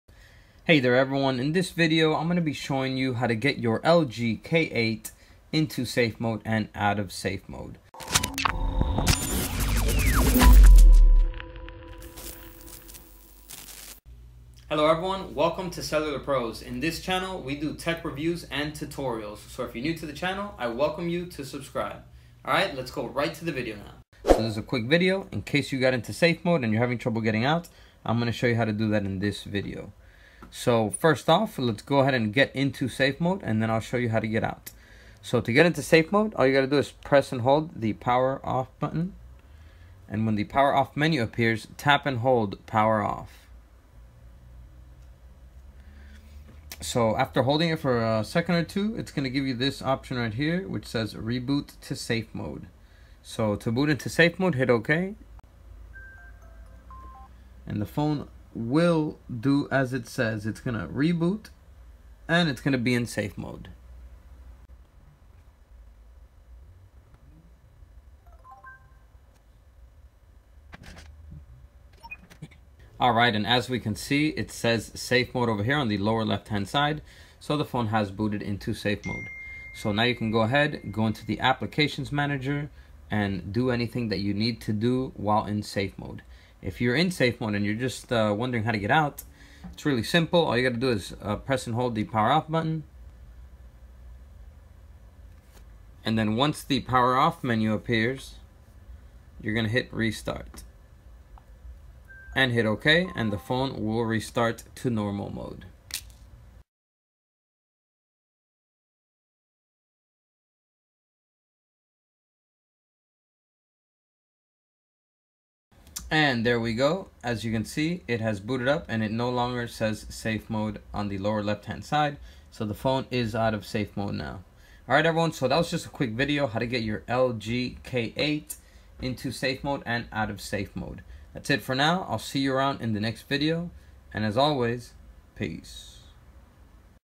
How to get LG K8 2018 IN & OUT of Safe Mode
This is a video on how to get a LG K8 2018 in and out of safe mode.

All models:
LG K8
LG K9
X210ULMG